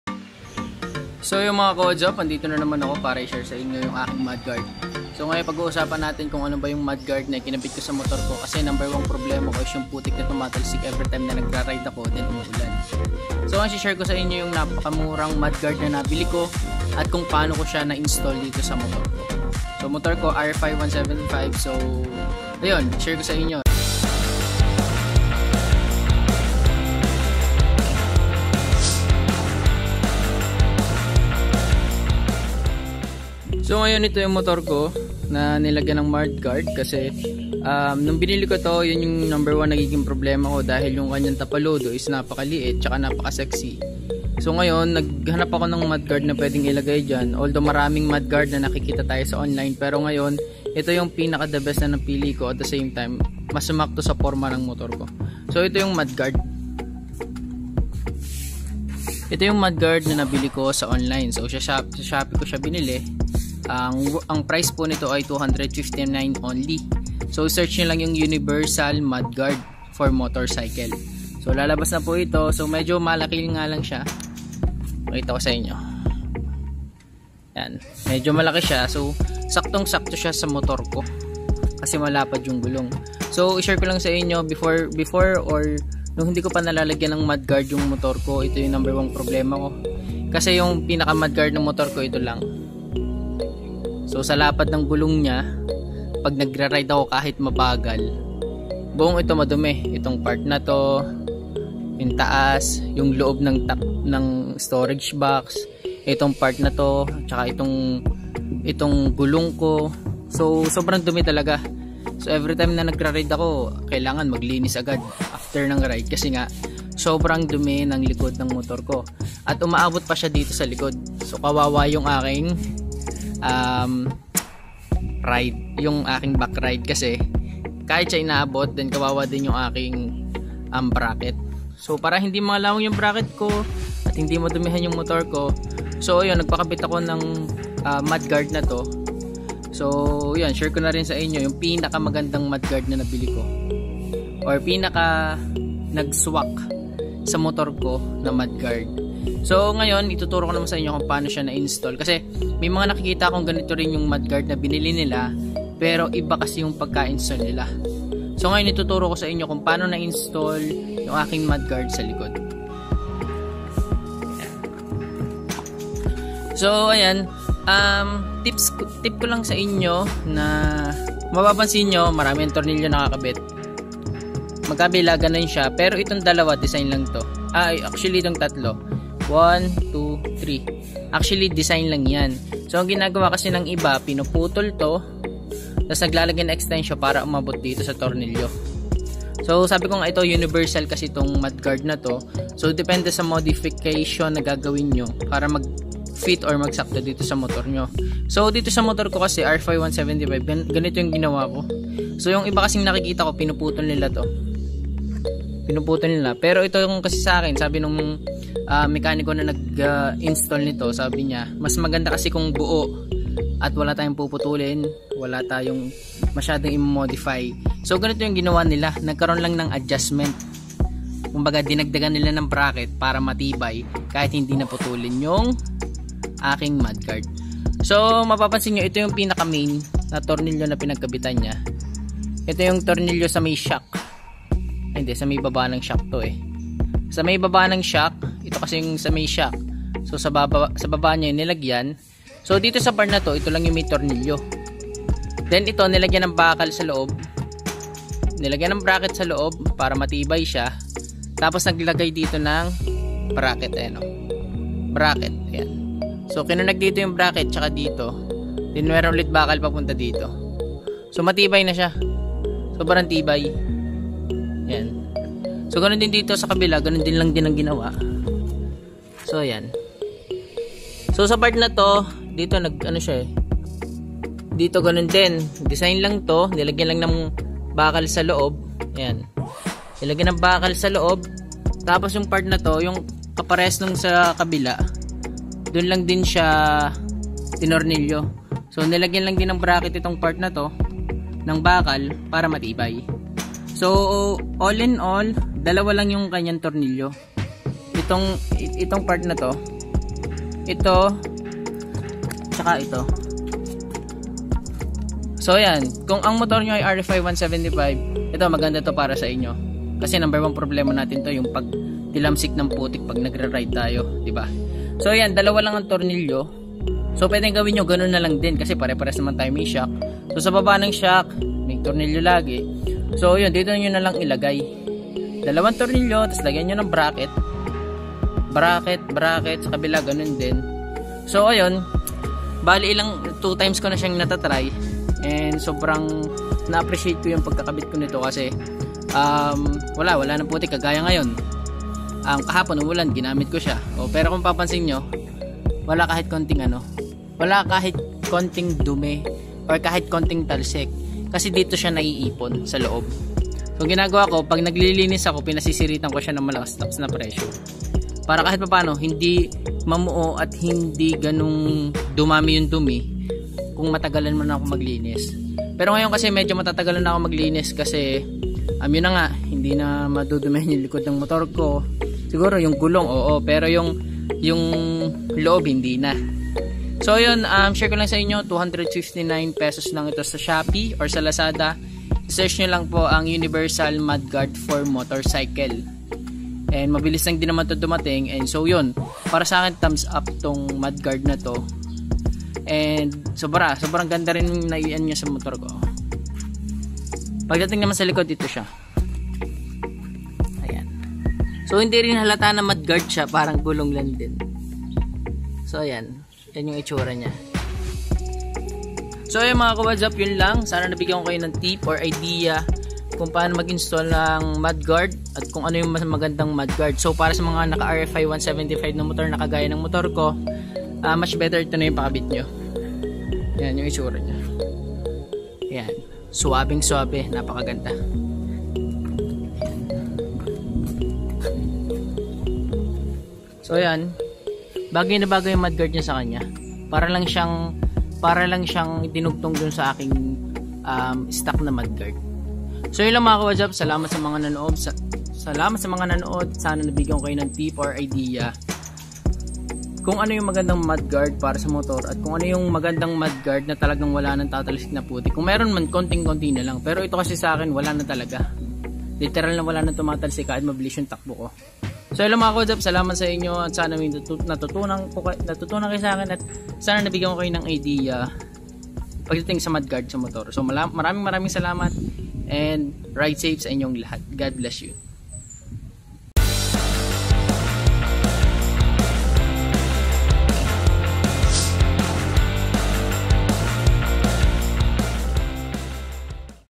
UNIVERSAL MUDGUARD FOR MOTORCYCLE | TIPS KUNG PAANO MAGKABIT | RFI 175
Share kolang po itong nabili kong universal mudguard sa Shopee sa halagang 259 pesos. Turo konadin kung paano ko siya pinakabit. Thanks and RS to all